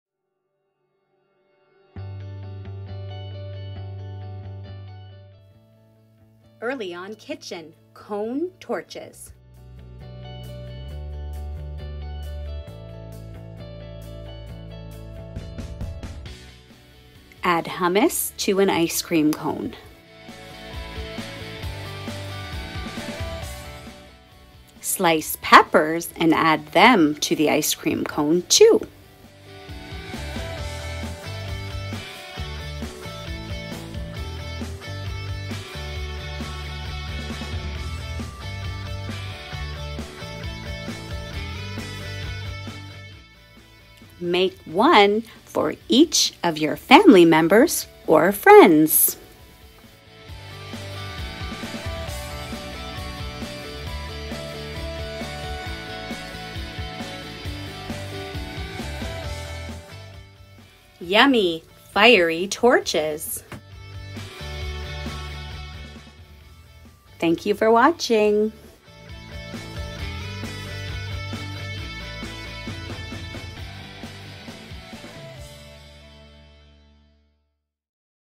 EarlyON Kitchen
EarlyON Kitchen: Cone Torches

Celebrate the Olympics with this healthy snack!